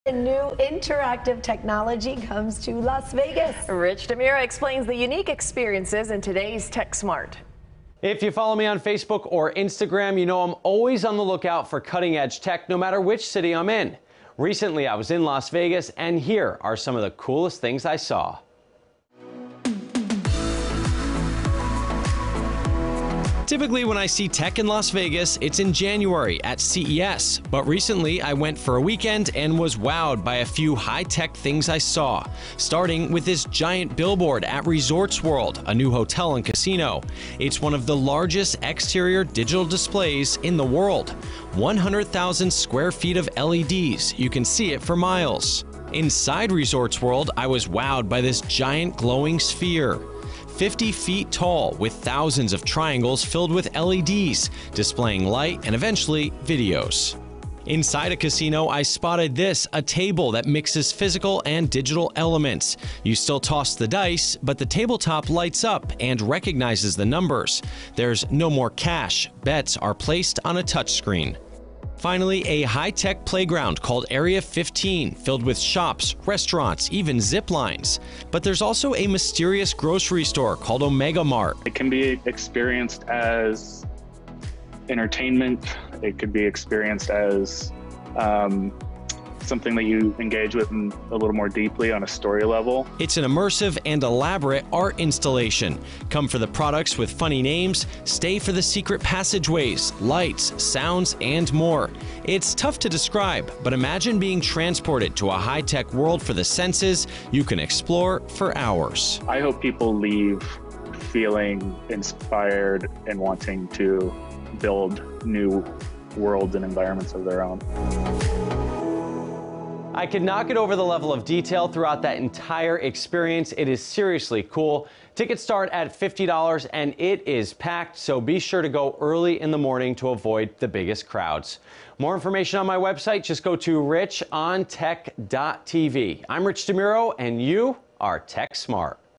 New Las Vegas Art Exhibit Features High-Tech Playground
There’s a relatively new grocery store in Las Vegas called Omega Mart and it’s not your typical market. It’s an elaborate art tech installation located just off the Strip at an even bigger high-tech playground called Area 15.